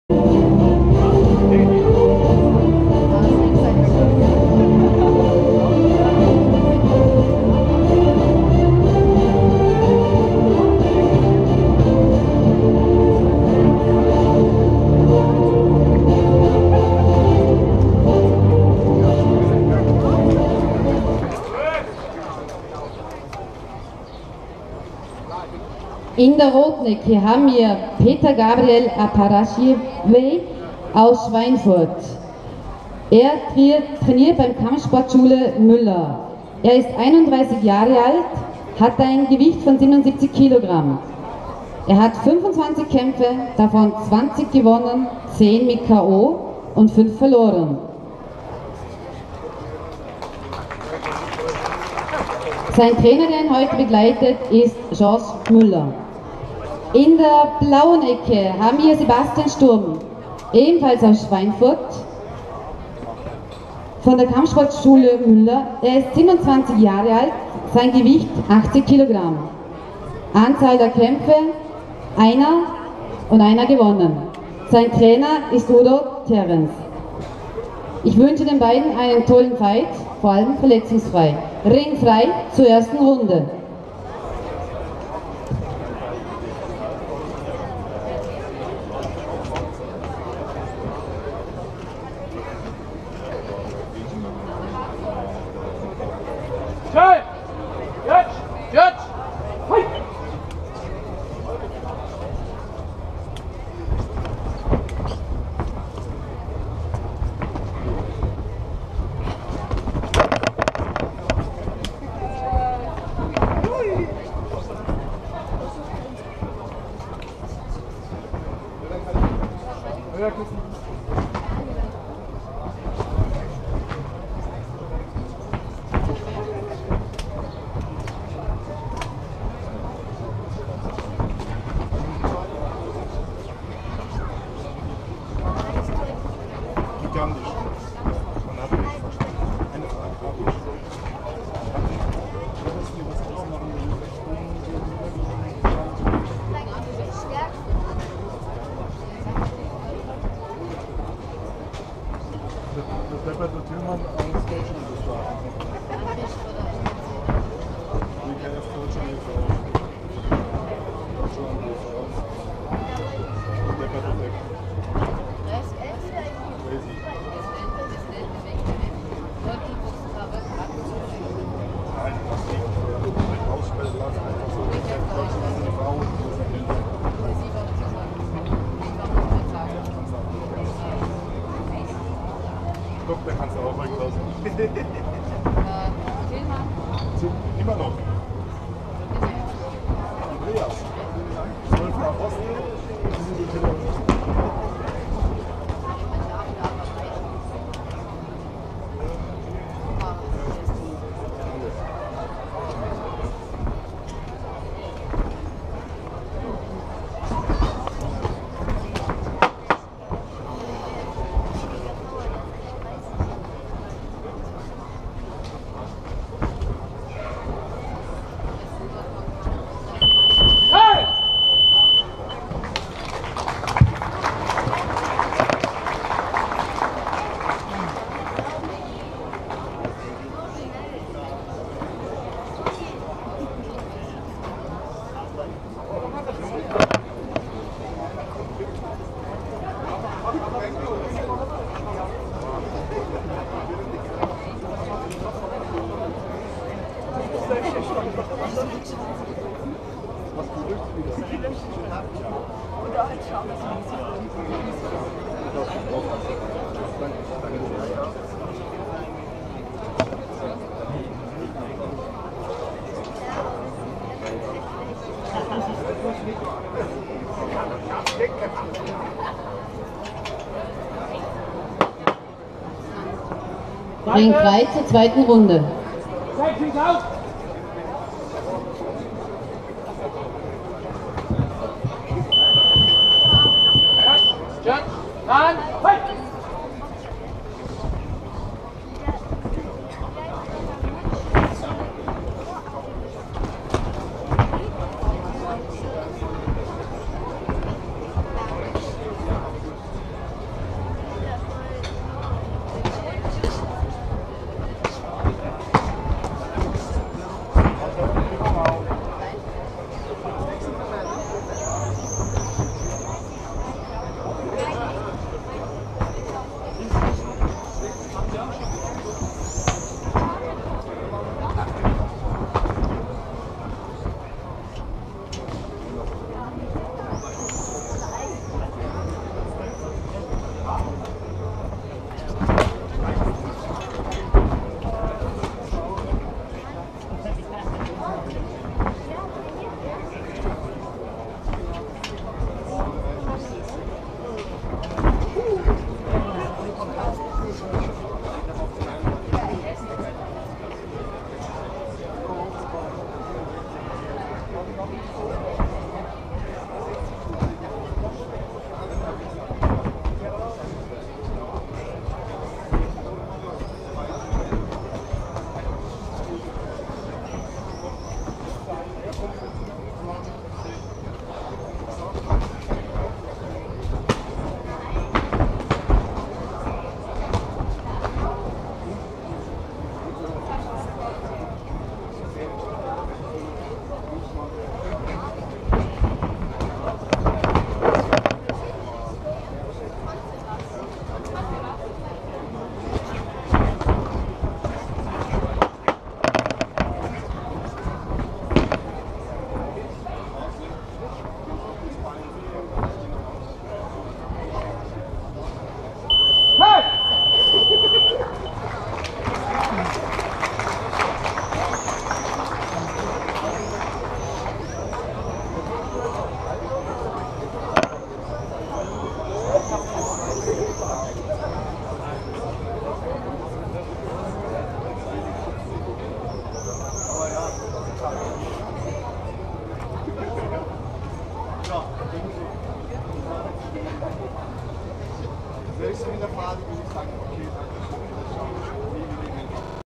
open-air Veranstaltung V - fight event V in Bregenz 19.07.2014
5.  beide Kämpfer aus der Kampfsportschule Müller - Petre-Gabriel Aparaschivei gegen Sebastian Sturm
Leider nur die erste und zweite Runde. Die dritte Runde fehlt. Der Sieger aus dem Kampf war eindeutig Petre-Gabriel Aparaschivei

5. both fighters from that fight sport school Müller - Petre-Gabriel Aparaschivei vs. Sebastian Sturm
Unfortunately, only the first and second round. The third round is absent. The winner from the fight was unambiguously Petre-Gabriel Aparaschivei